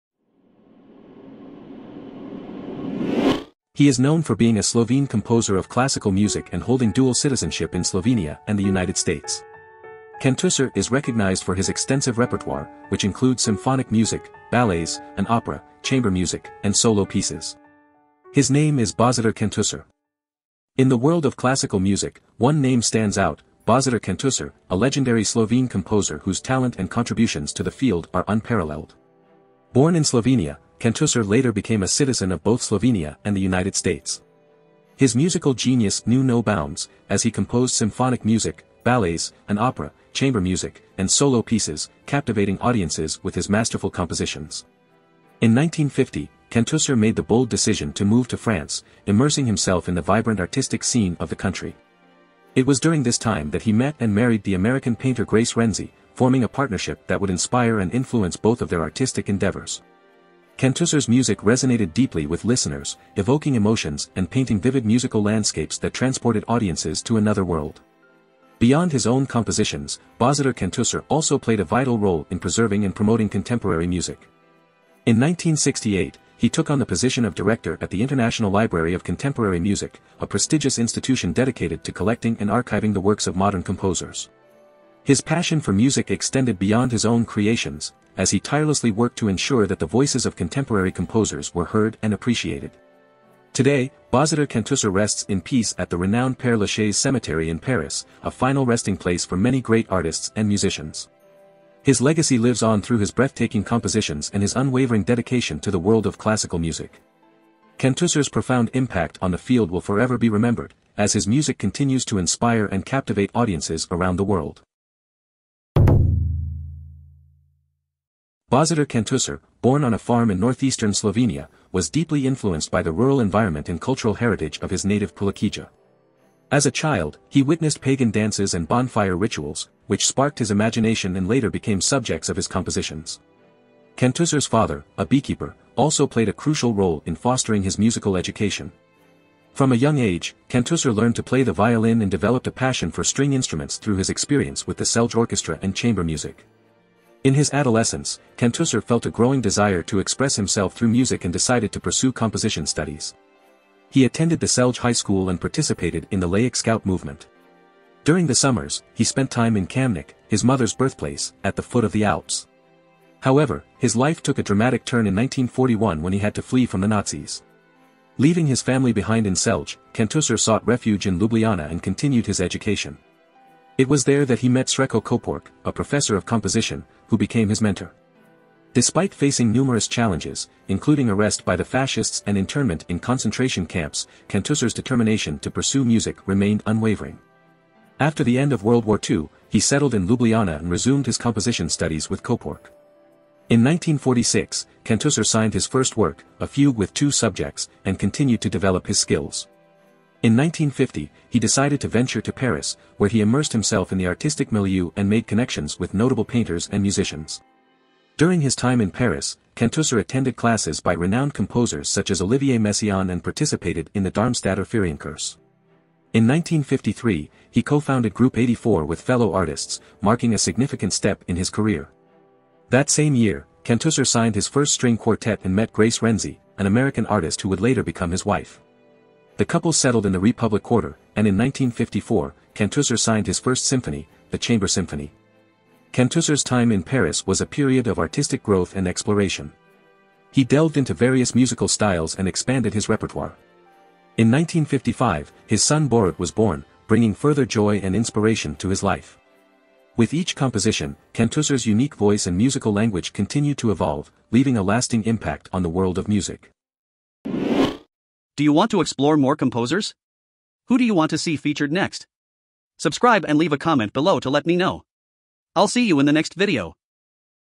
Božidar Kantušer: Harmonic Maestro | Composer & Arranger Biography
Božidar Kantušer was a Slovene composer of classical music. He was a Slovenian citizen and an American citizen.
Kantušer is the author of symphonic music, ballets, an opera, chamber music and solo pieces. He lived in France since 1950. He was married to the American painter Grace Renzi. Since 1968, Kantušer was the director of the International Library of Contemporary Music . He is buried in Paris, at the Père Lachaise Cemetery.

456e